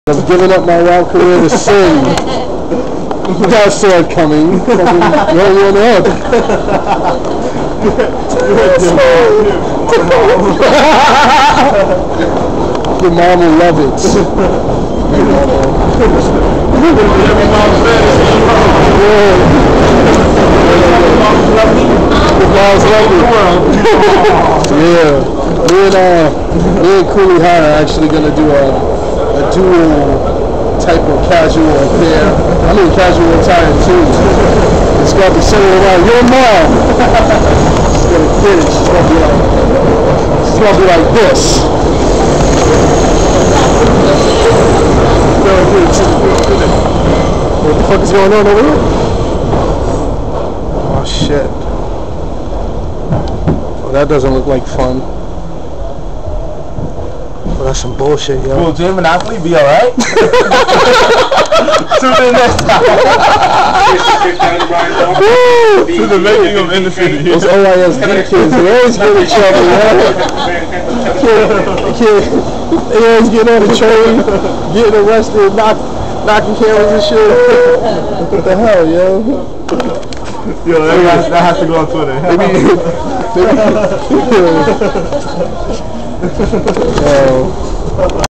J.Monop gets harrassed by Cops...AGAIN!
"Will J. Monopoly be alright?" "Tune in next time!"

More police mayhem on the set of in the city!